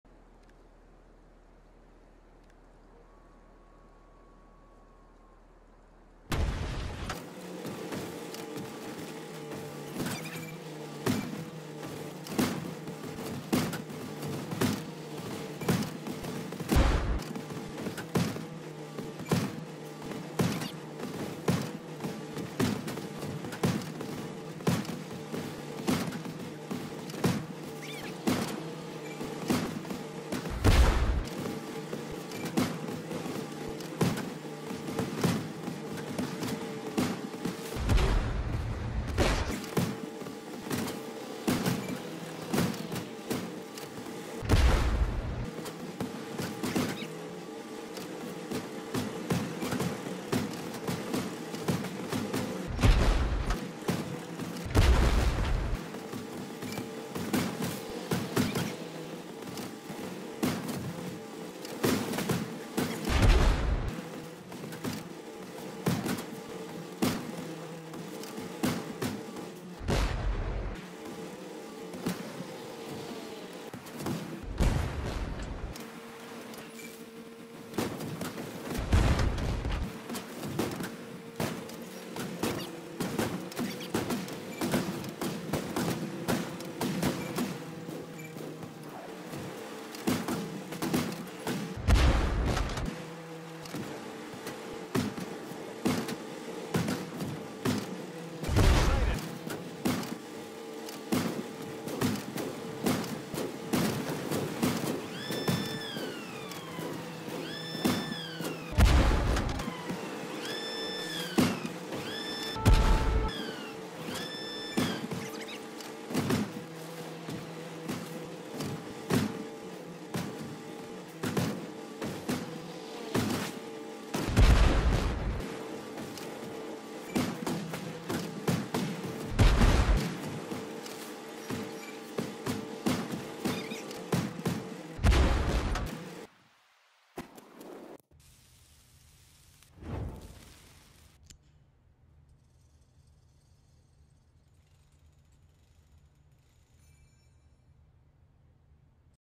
SPANKED 'EM - 17 TIMES
GOOD BATTLE 06-11-23  The video was 11 minutes but I sped up all but my kill shots. The bigger boom you hear each time, is the tank firing the round (tank exploding, isn't as loud).